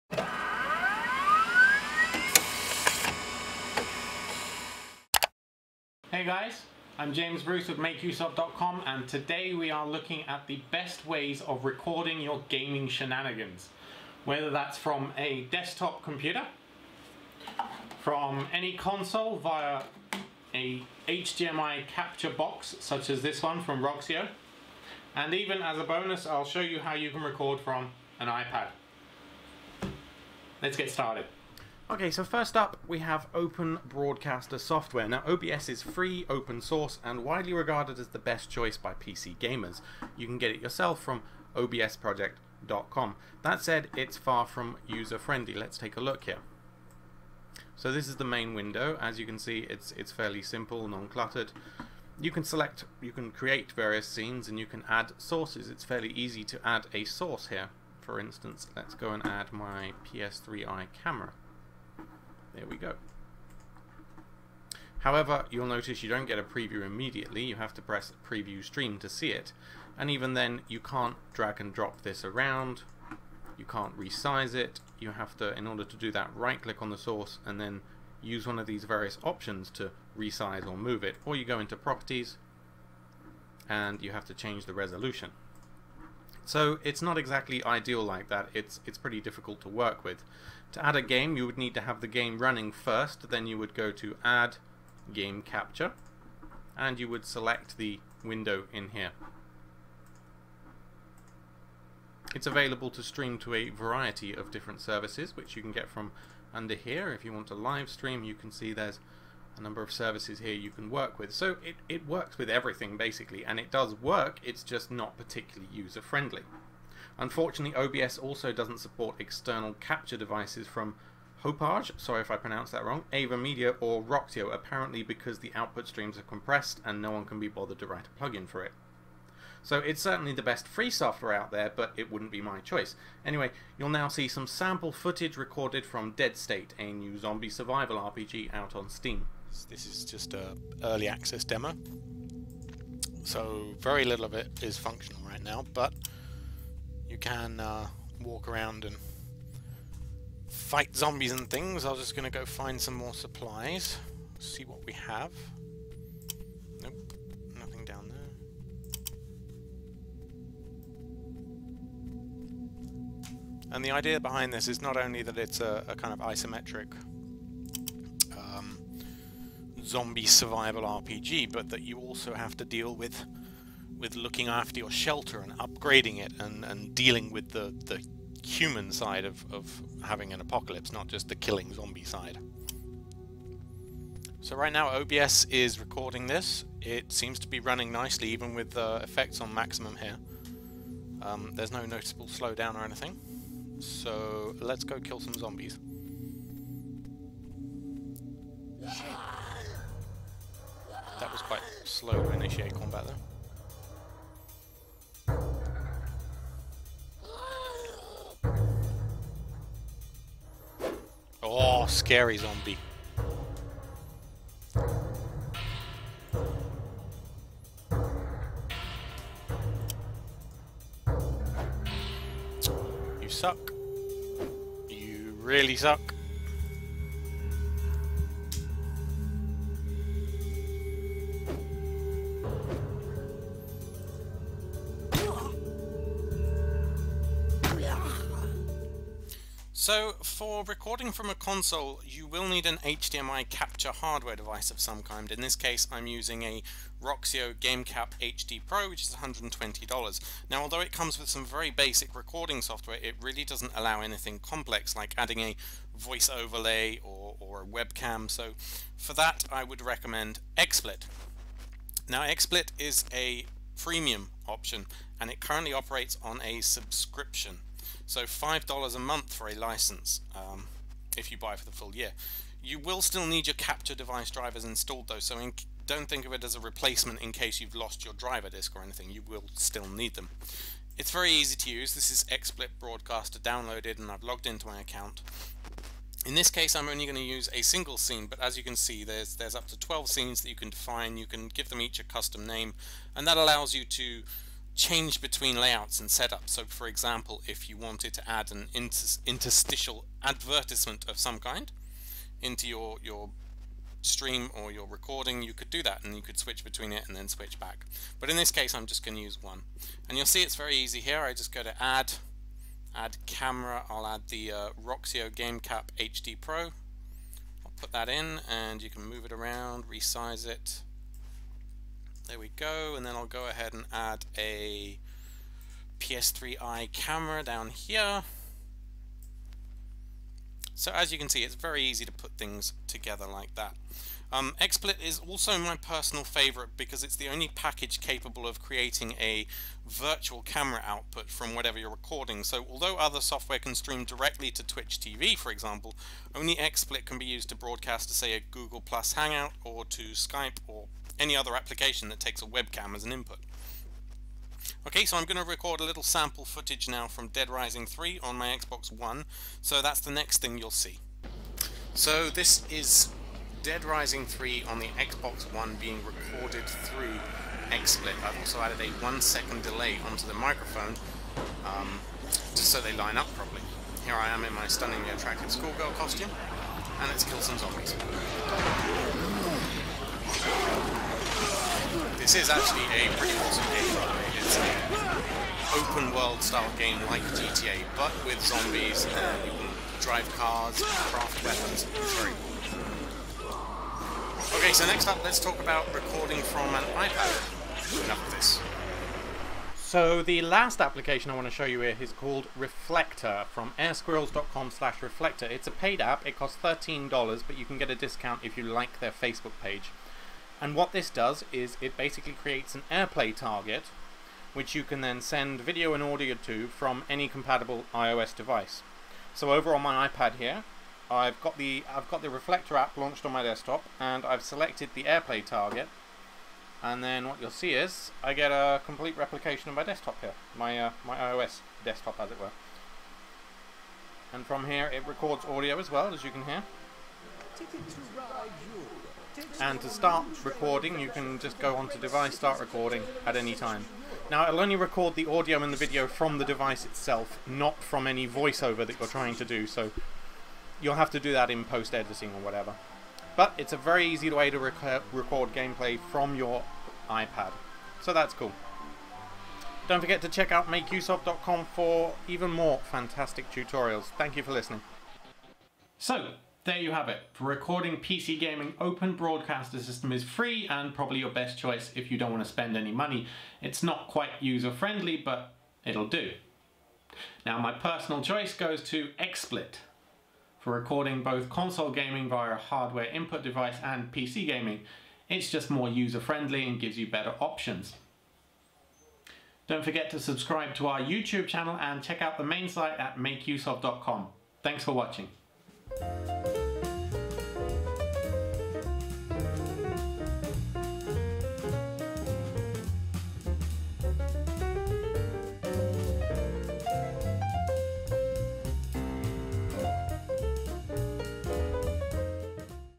Open Broadcaster Software, Xsplit, and Reflector For Game Capture Tested
Gaming is awesome: but sharing that with the world is even better. To do that, you'll need some way of recording your antics. In today's video, I'll show the best way to record from a PC, a console, and even from an iPad.